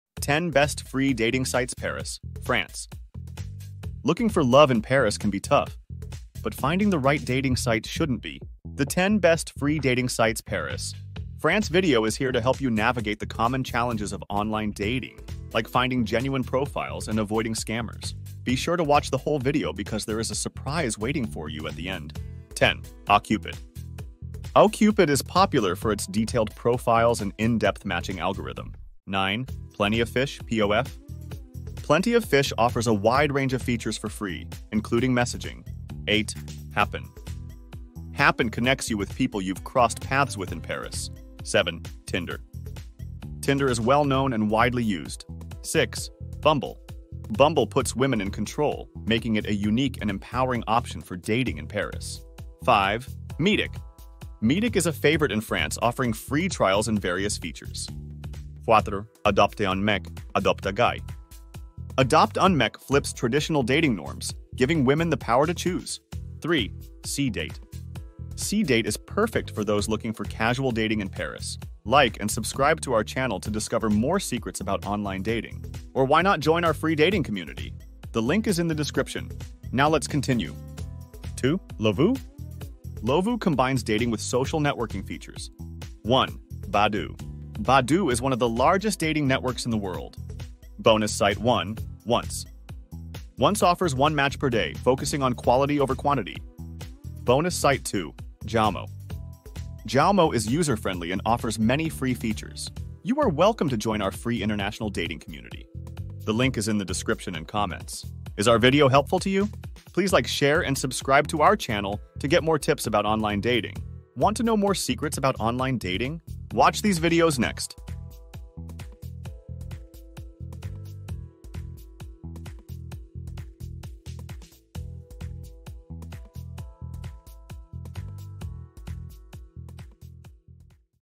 ❤️10 BEST ►FREE◄ Dating Sites Paris France 2024
10 BEST ►FREE◄ Dating Sites Paris   France

Looking for love in Paris can be tough, but finding the right dating site shouldn't be. The 10 Best ►FREE◄ Dating Sites Paris - France video is here to help you navigate the common challenges of online dating, like finding genuine profiles and avoiding scammers.

Be sure to watch the whole video, because there is a surprise waiting for you at the end.

#10: **OkCupid**
OkCupid is popular for its detailed profiles and in-depth matching algorithm. It’s perfect for finding serious relationships in Paris without spending a dime.

#9: **Plenty of Fish (POF)**
Plenty of Fish offers a wide range of features for free, including messaging. It's great for meeting new people in Paris with minimal hassle.

#8: **Happn**
Happn connects you with people you’ve crossed paths with in Paris. It’s ideal for turning those missed connections into potential dates.

#7: **Tinder**
Tinder is well-known and widely used. It’s fantastic for meeting new people quickly and easily in Paris with its simple swipe mechanism.

#6: **Bumble**
Bumble puts women in control, making it a unique and empowering option for dating in Paris. It’s great for those looking for a safer and more respectful dating experience.

#5: **Meetic**
Meetic is a favorite in France, offering free trials and various features. It’s excellent for serious daters who want to meet like-minded singles in Paris.

#4: **AdopteUnMec (AdoptAGuy)**
AdopteUnMec flips traditional dating norms, giving women the power to choose. It’s a fun and free way to find potential partners in Paris.

#3: **C-Date**
C-Date is perfect for those looking for casual dating in Paris. Its free features and wide user base make it easy to find casual connections.

Do you like our tips so far? Or why not join our Free Dating Community? The link is in the description. Now let's continue:

#2: **Lovoo**
Lovoo combines dating with social networking features. It’s great for meeting new people in Paris through its live video and discovery features.

#1: **Badoo**
Badoo is one of the largest dating networks in the world. It offers many free features and is excellent for meeting singles in Paris with its extensive user base.

**Bonus Site 1: **Once**
Once offers one match per day, focusing on quality over quantity. It’s ideal for those who prefer a more deliberate and thoughtful approach to dating in Paris.

**Bonus Site 2: **Jaumo**
Jaumo is user-friendly and offers many free features. It’s perfect for those new to online dating in Paris and looking for a straightforward experience.

Surprise! You are welcome to join our Free International Dating Community. The link is in the description and comments.

Video made with Pictory AI Video Creator